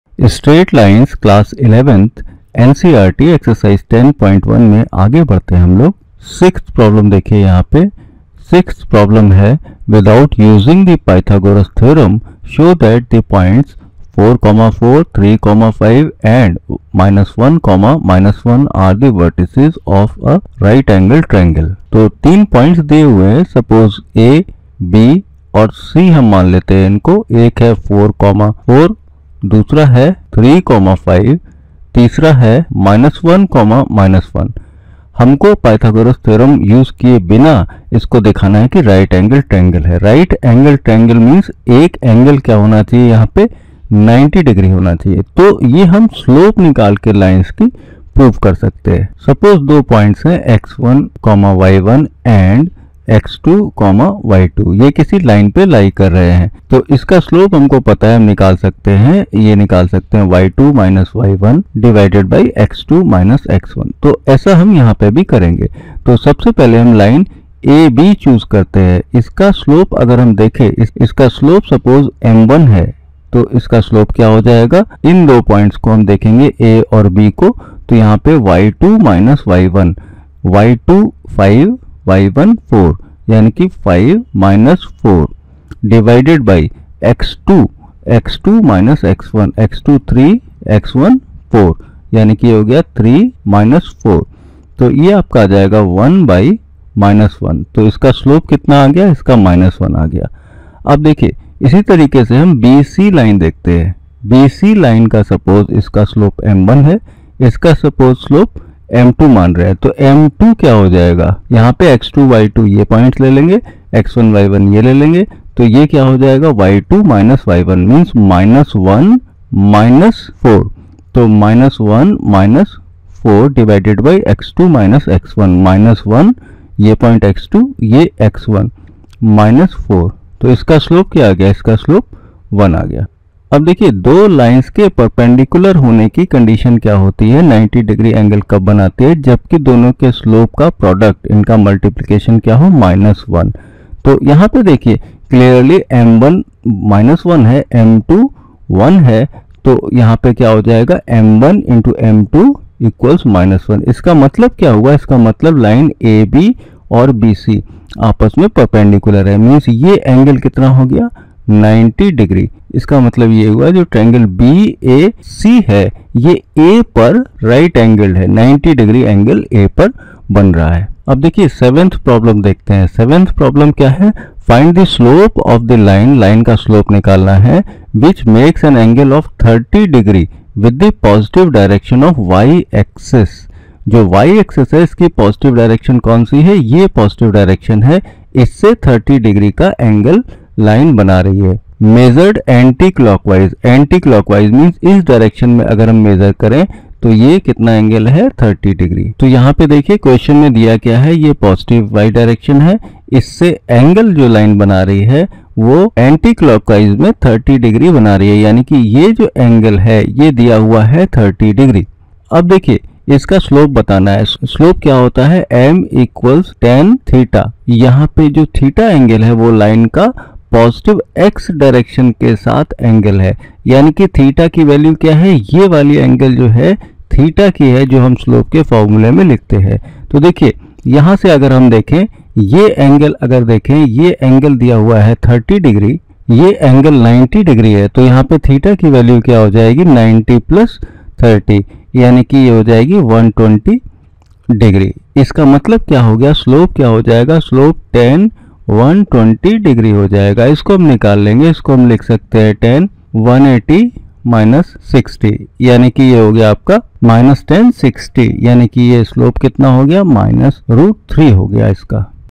Straight Lines : Class 11 / NCERT || Exercise 10.1 || Q.6 & Q.7
Explanatory solutions of questions 6 and 7, Exercise 10.1, Straight Lines, NCERT, Class 11.